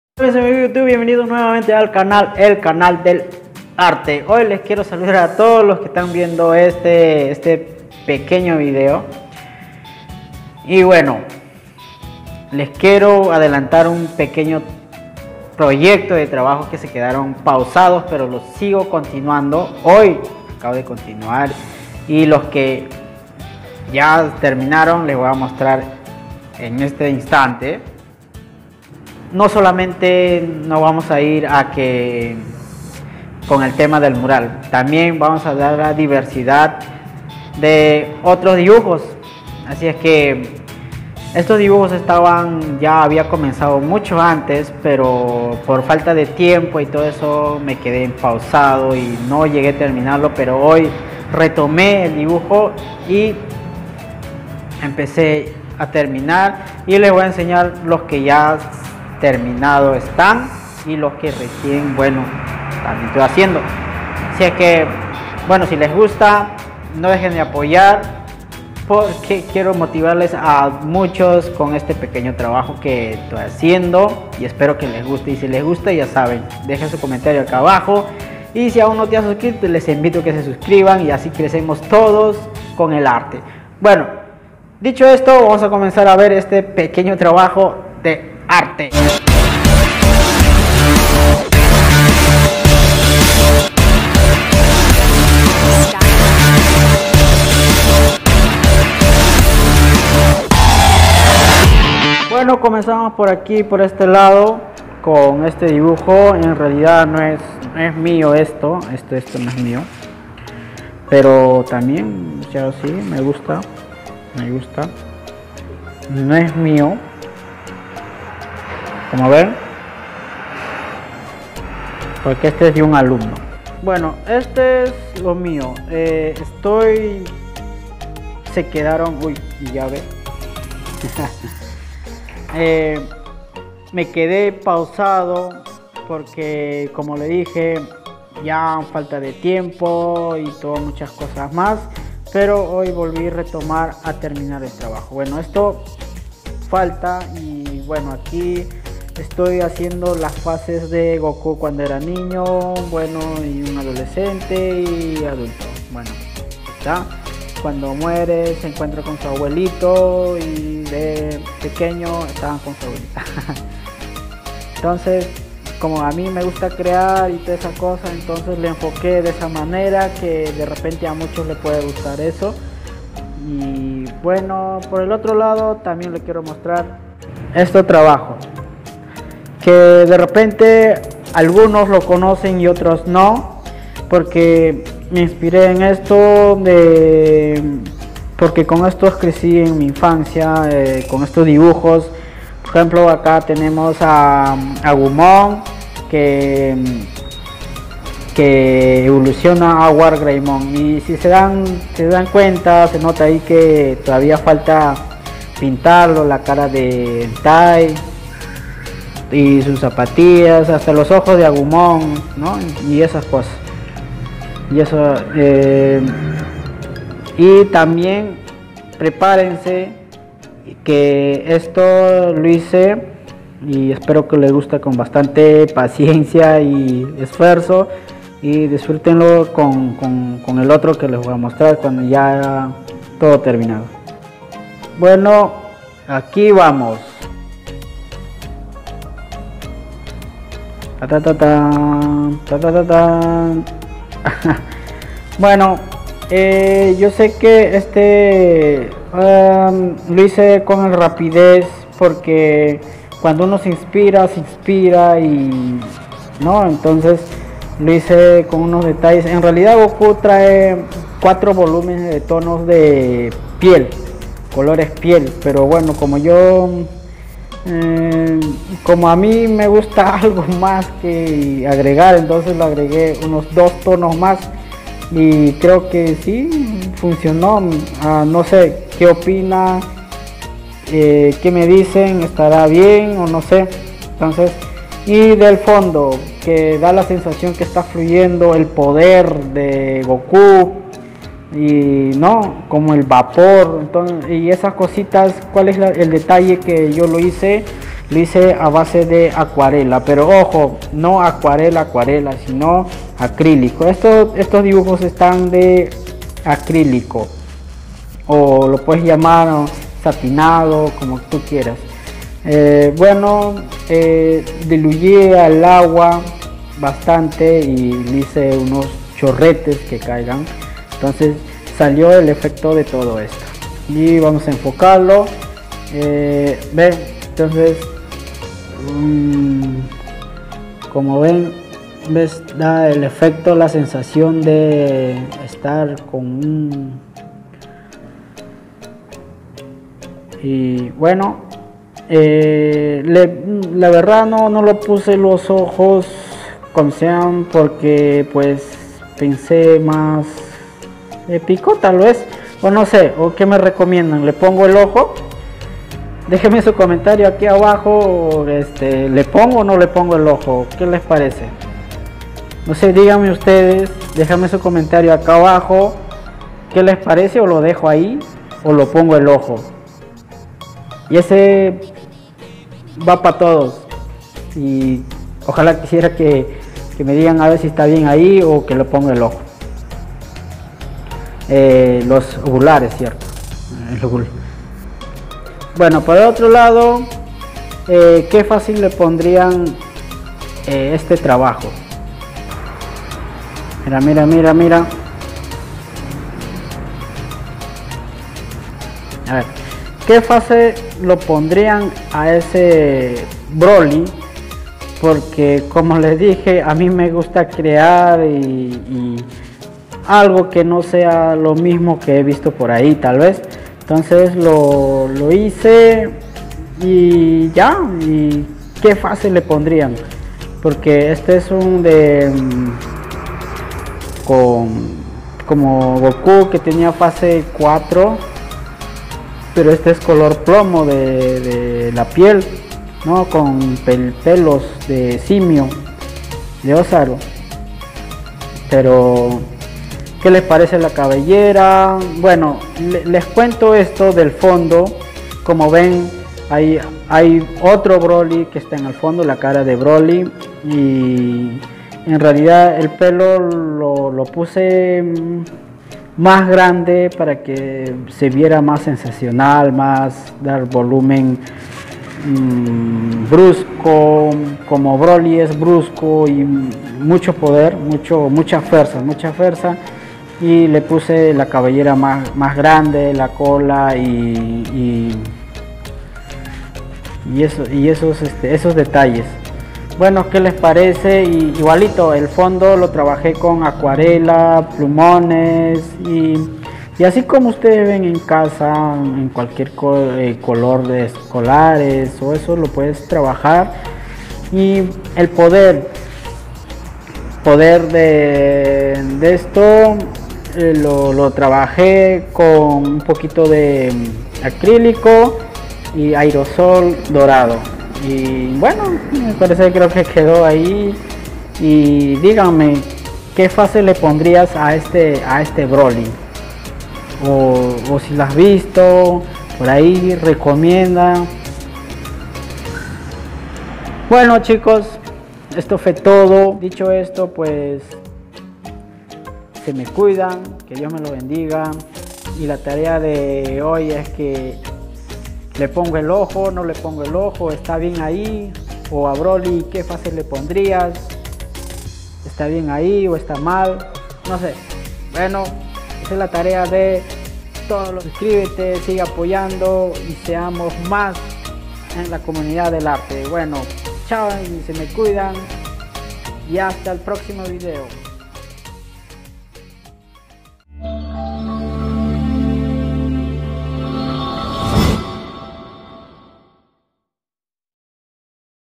GOKU Y BROLY EN SU TRANSFORMACIÓN FACE UNICO
Goku y broly en su face único pintado a base de pintura acrílica y técnicas de acuarela y spray dorada
Que face lo pondría a broly ?
Déjame tu respuesta aquí en la caja de comentarios

Estos proyectos de dibujo es solo el avance porque se  viene más  crearemos los dibujos y faces únicos que nadies lo tiene ningún pagina
 5afKeqZ9I0Quedes seguirme  en mis redes: